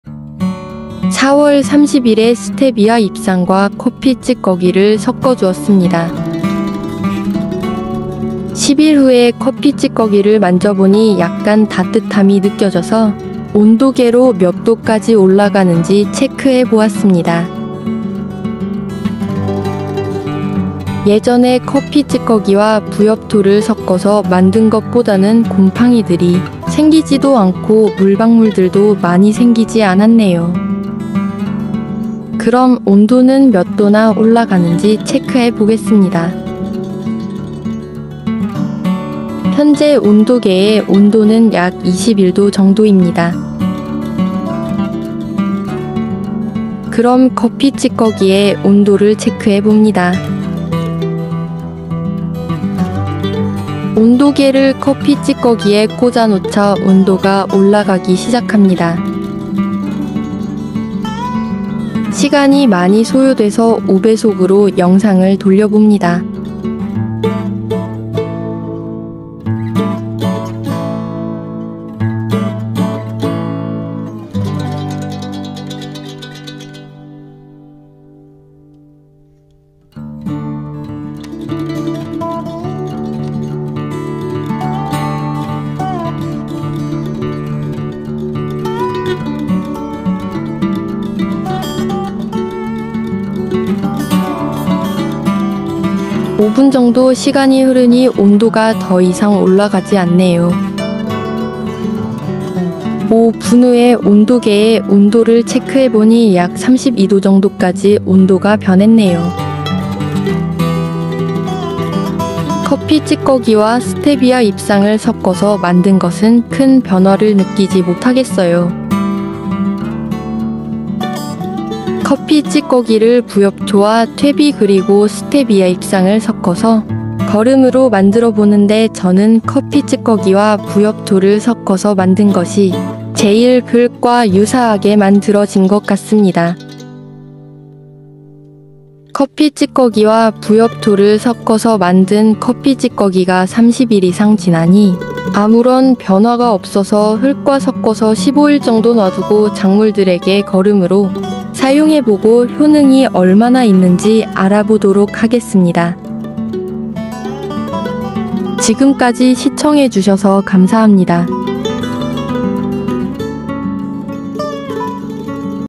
10일동안 발효시킨 커피찌꺼기에 온도는?거름 비료로 사용하려면 오이,양배추,상추,파프리카,고추,방울토마토,감자,대파등 거름으로사용. 순치기,
10일동안 발효시킨 커피찌꺼기에 온도는?